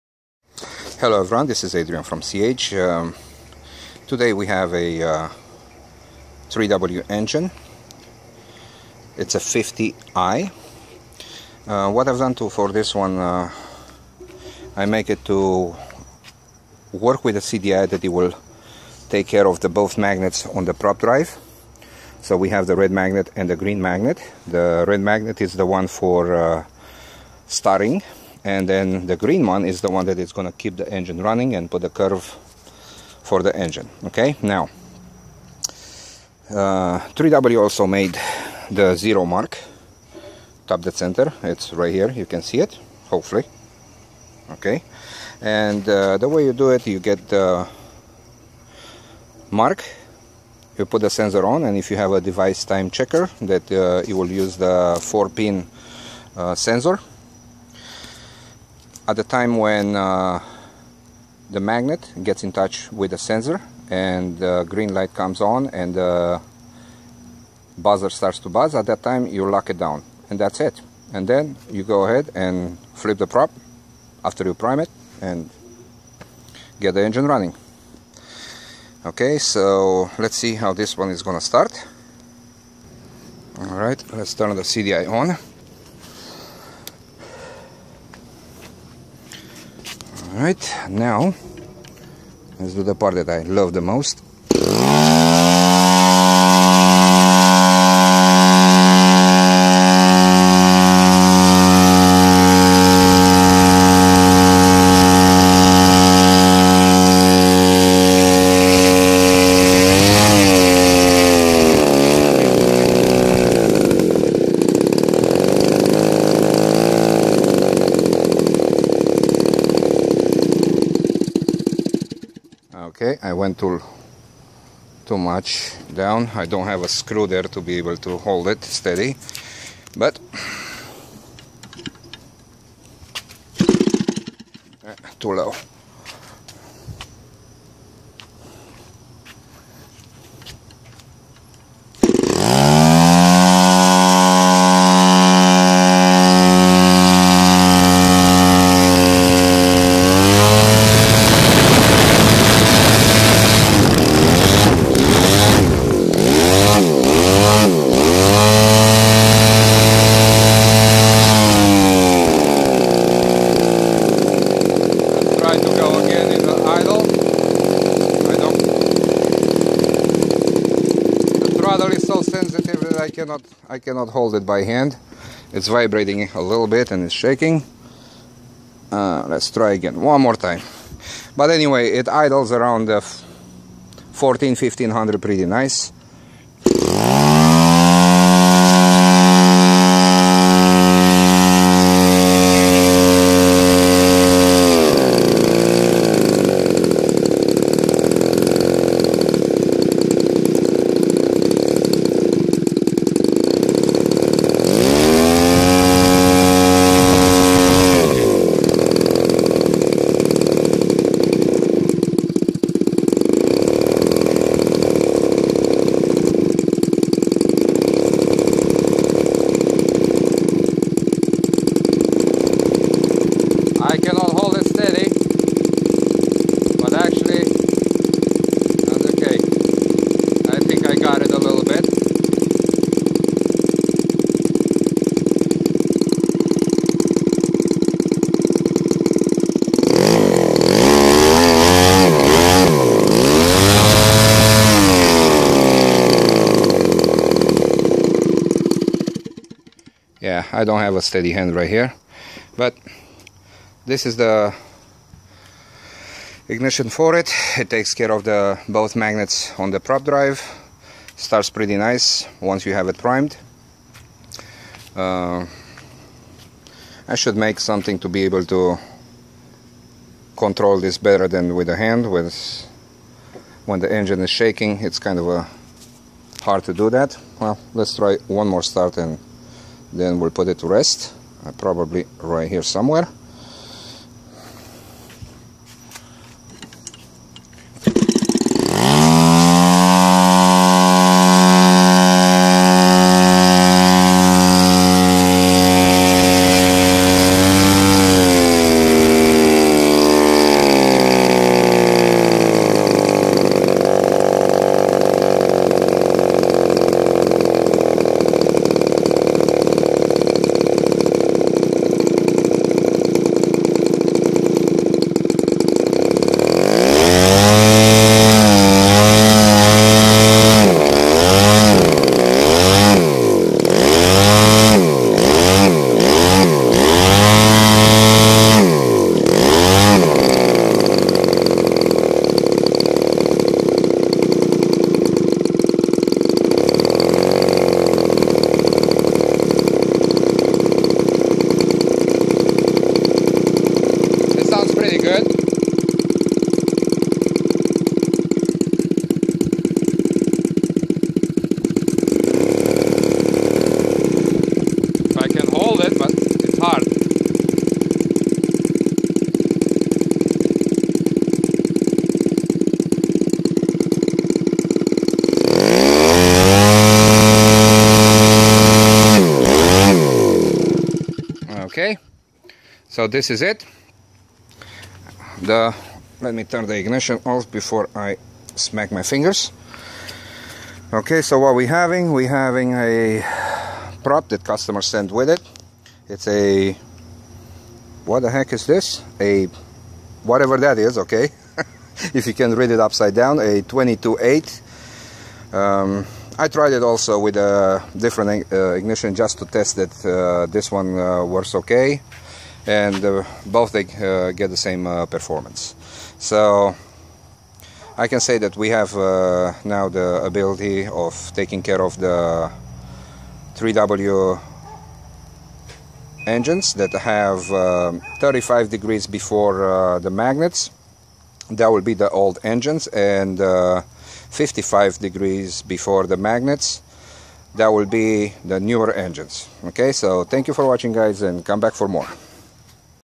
3W 50i, Running On Dual Sensor CDI by CH IGNITION
Engine is running  on a cdi that uses  both magnets on the prop drive.